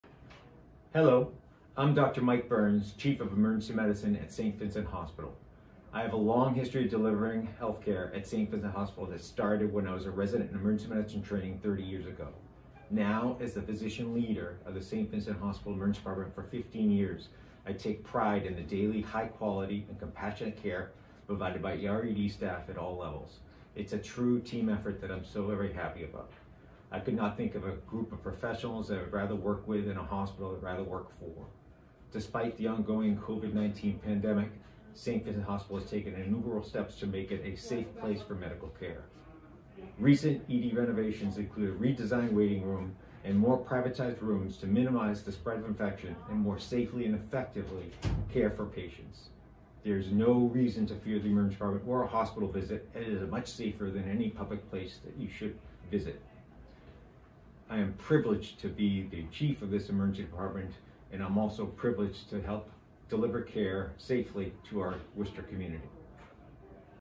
Dr. Michael Burns, Chief of the Saint Vincent Hospital Emergency Department
Dr. Michael Burns, Chief of the Saint Vincent Hospital Emergency Department shares his experience working at Saint Vincent Hospital.